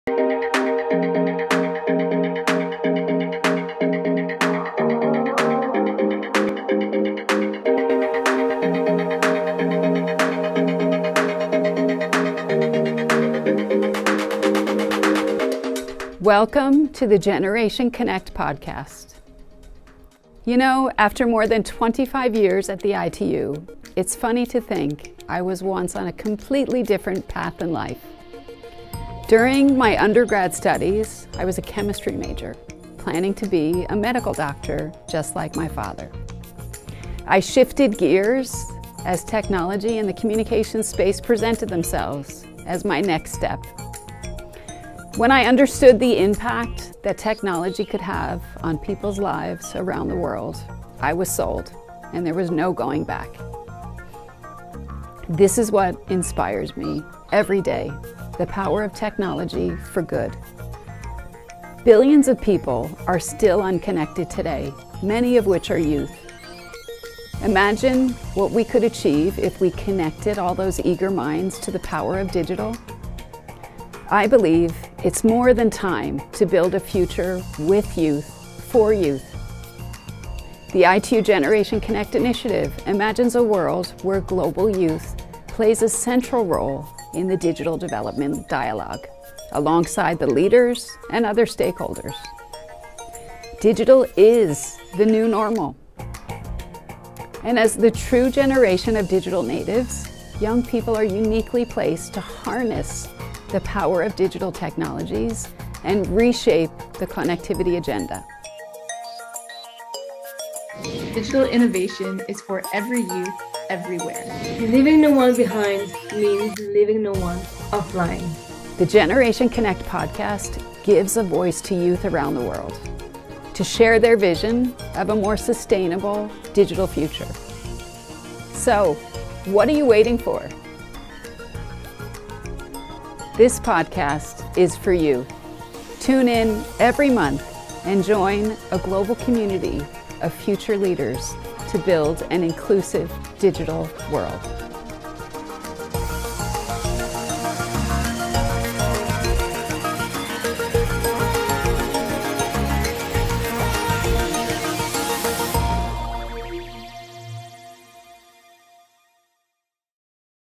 Teaser for the Generation Connect Podcast
The Generation Connect podcast is fully designed by youth and for youth. It is part of the Generation Connect Initiative of the International Telecommunication Union (ITU) which aims to engage global youth and encourage their participation as equal partners alongside the leaders of today’s digital change.

By covering topics that are relevant to youth, the Generation Connect Podcast seeks to give youth around the world a platform to speak up and share their vision of a more sustainable digital future.